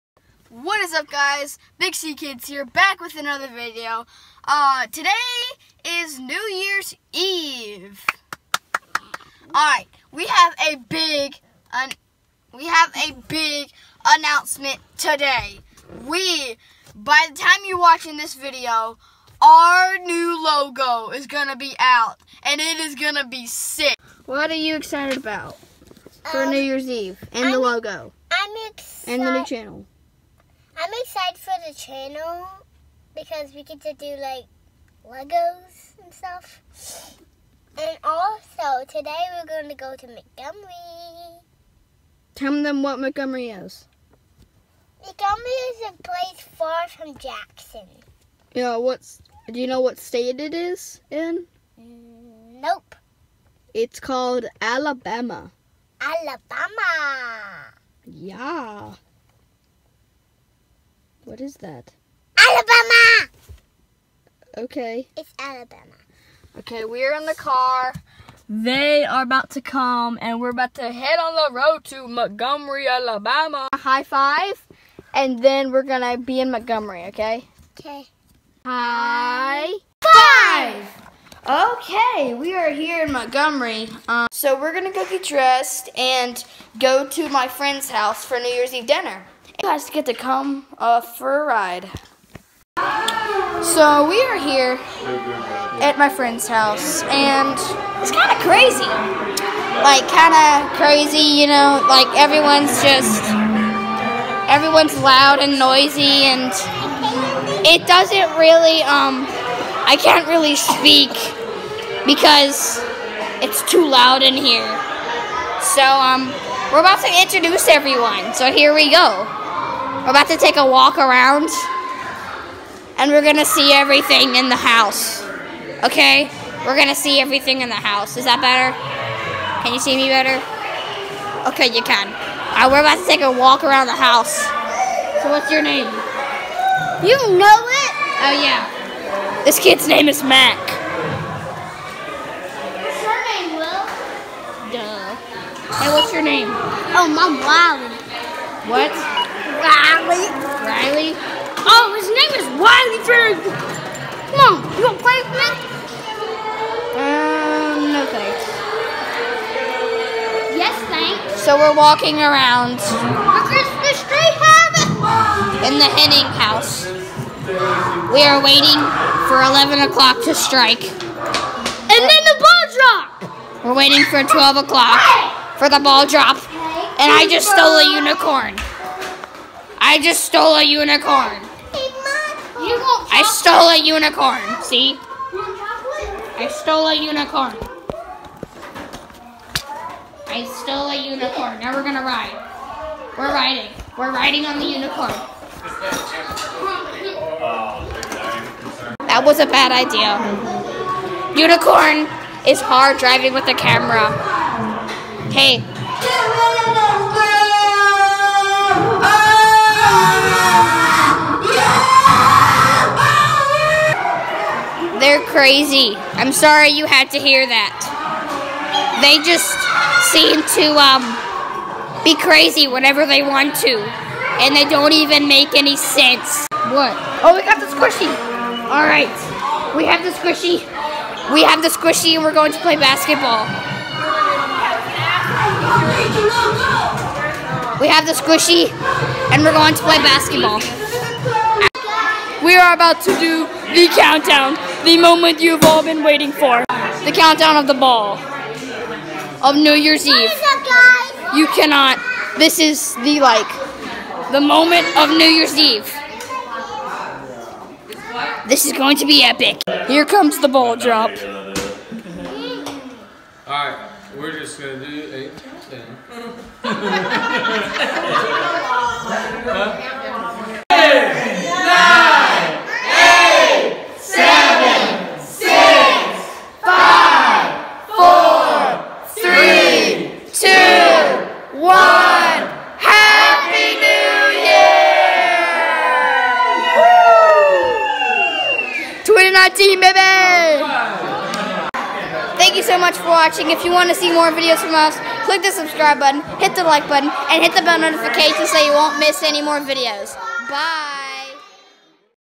Happy New Year!!
We want to wish you a Happy New Year!  Big things are in the works for the Big C Kids so we hope you will keep coming back in 2019 to enjoy the ride.